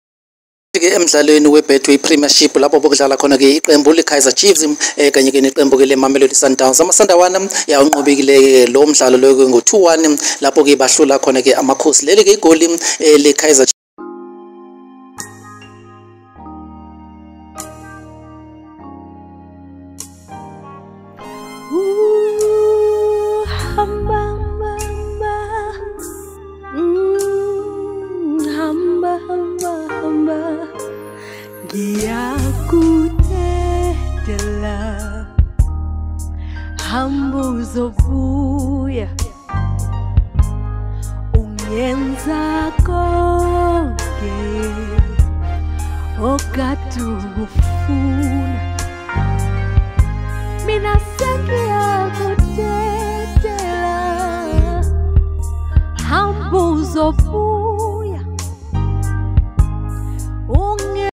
Kaizer Chiefs goal disallowed
In the Betway premiership match at FNB Stadium Chiefs up against Sundowns.